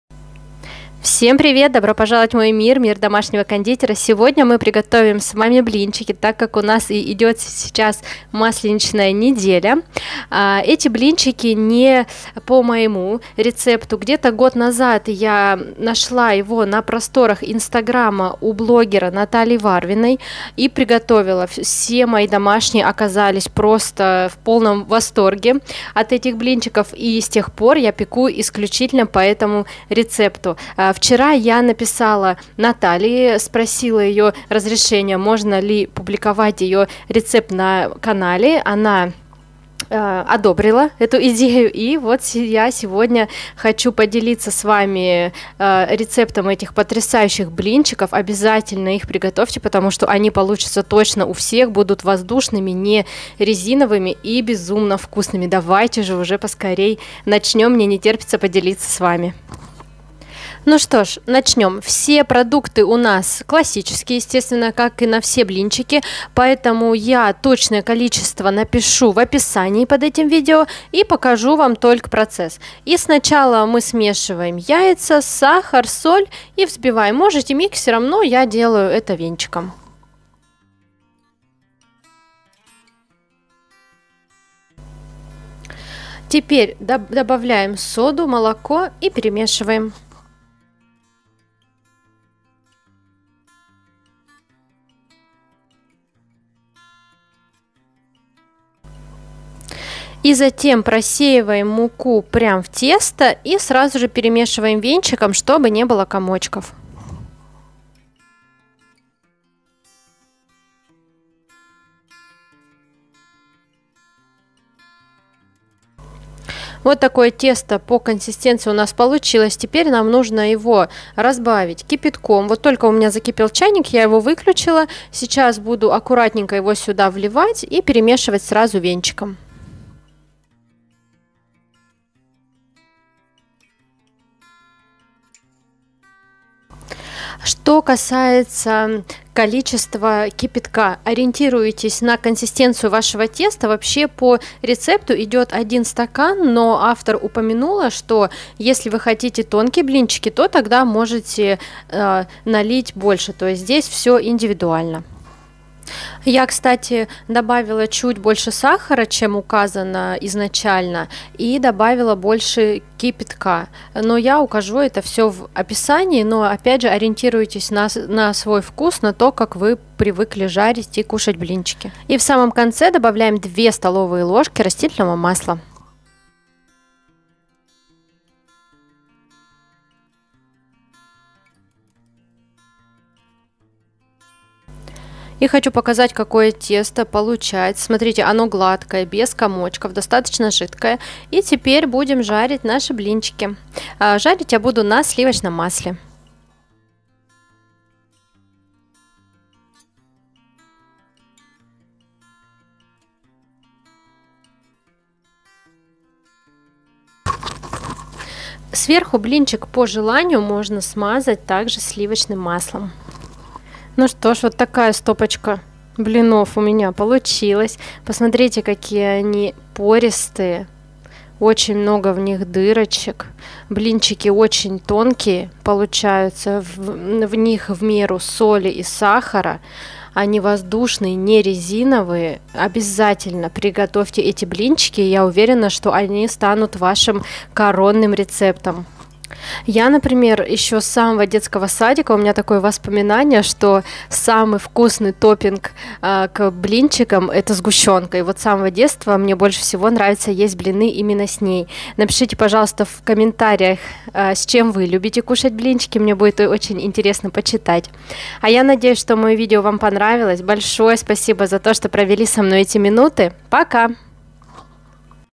Самые вкусные, пористые и тонкие блинчики. Идеальный рецепт.
В этом видео я расскажу и покажу как приготовить самые вкусные блинчики.  Уверяю вас, попробовав один раз, вы будете готовить только по  этому рецепту.

🔷 Ингредиенты

- яйца С0 - 2 шт.
- сахар - 2 ст.ложки  ( или по вкусу,  я добавила 4 ст.ложки)⠀
- соль - 1 ч.ложка
- сода - 1 ч.ложка ( только не полная )
- кипяток - 250 мл. ( для тонких блинчиков добавляйте побольше)